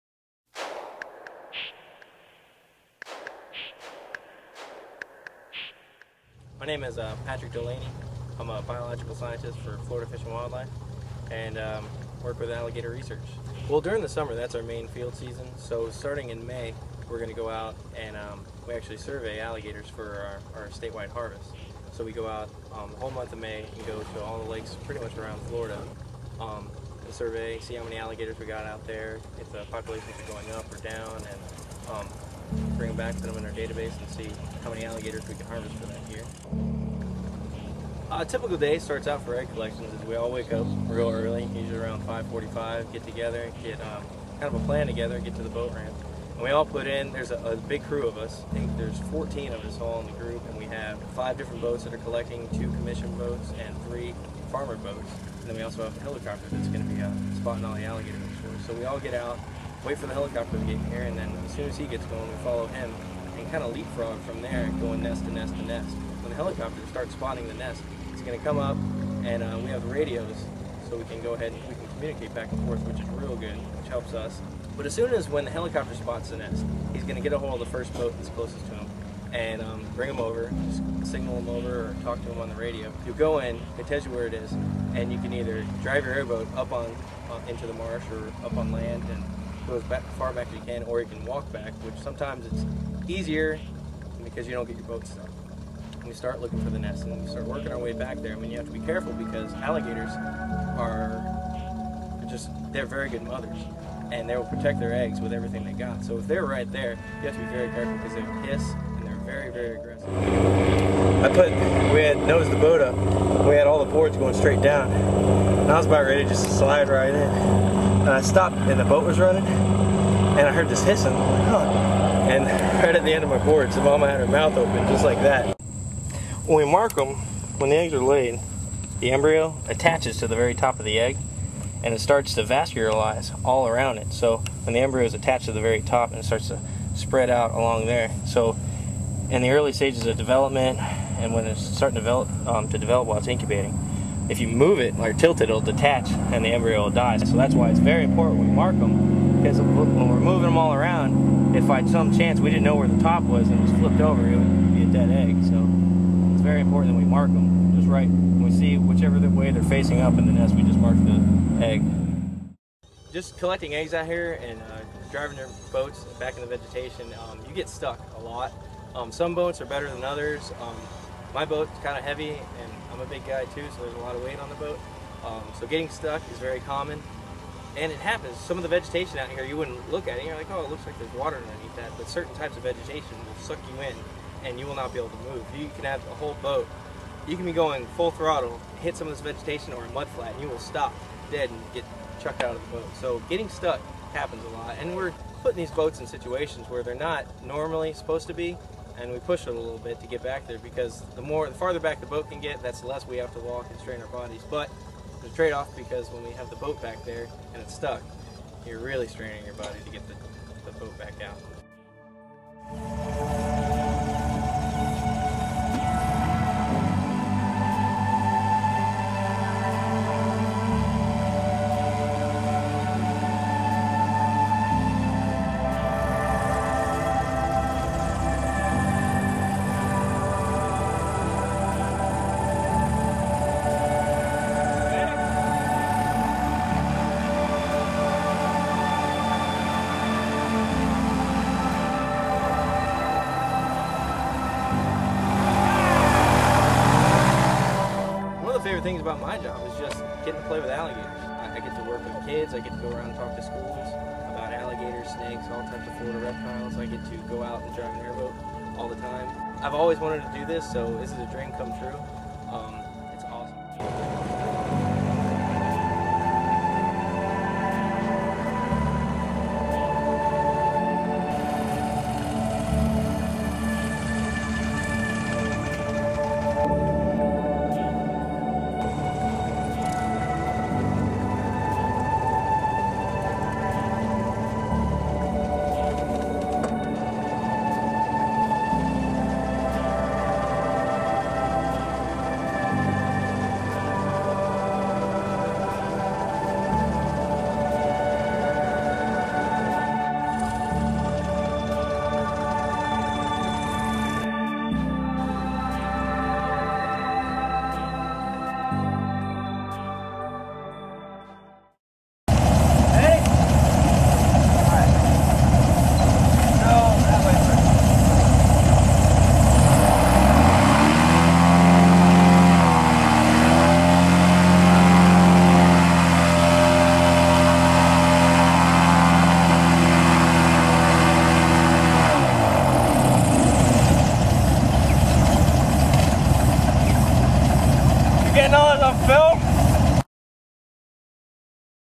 FWCC Patrick Delaney Alligator Egg Collection 2007
A day in the life of biologist Patrick Delaney.
FWCC alligator egg collection 2007.